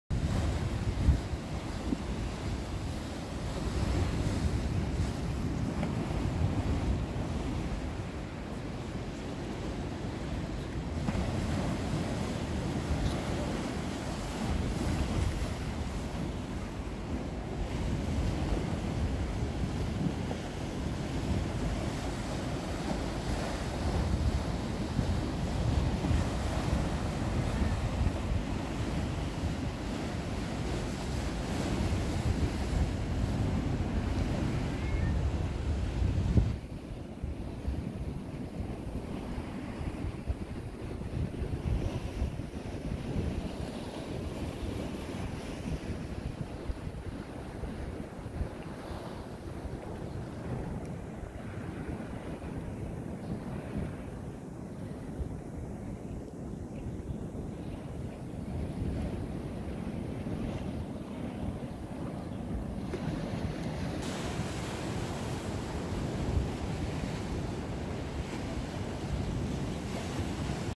Birds by the sea: European herring gulls a quick footed ruddy turnstone (Arenaria interpres)
The gulls were chilling by the sea, when a little ruddy turnstone came by with its quick little feet.
You can see the difference in the look of the gulls: one is a mature one and the other a juvenile (the one with darker, browner plumage).